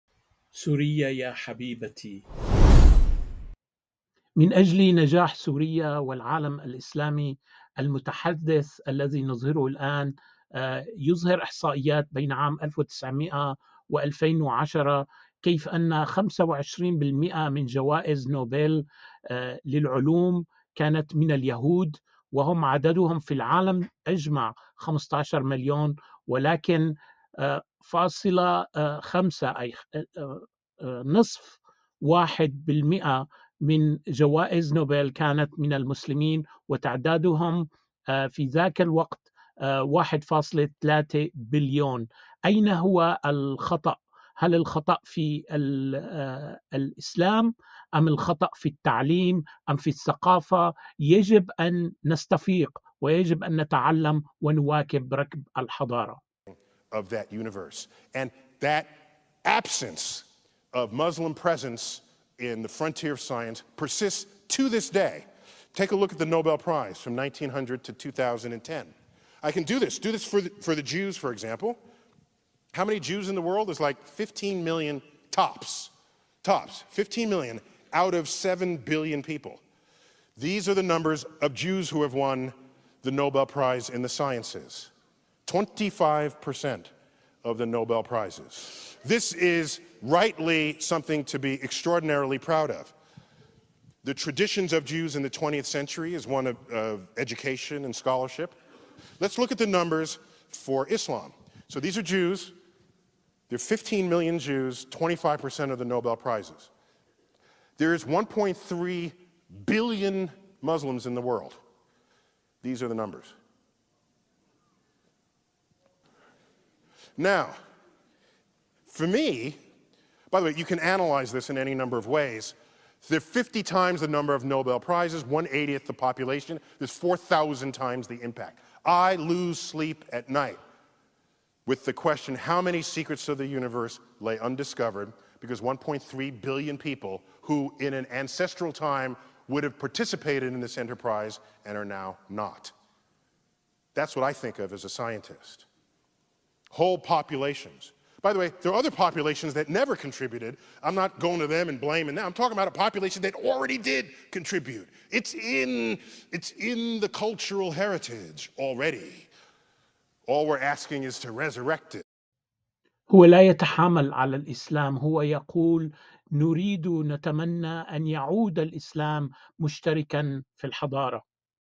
225 فقه المقاصد يعيد الاسلام ليشترك في الحضارة
بين عامي 1900 و 2010 عدد اليهود في العالم 15 مليون حصل 150 منهم على جوائز نوبل في العلوم أي 25% من الجوائز

أما المسلمون و تعدادهم 1.3 مليار (87 ضعف اليهود) حصلوا على 3 جوائز فقط أي 0.5% من جوائز نوبل في العلوم، واحد باكستاني بريطاني (فيزياء)، واحد مصري أمريكي (كيمياء)، و واحد من دولة مسلمة لكن أمريكي

كيف نعيد بلادنا للنجاح؟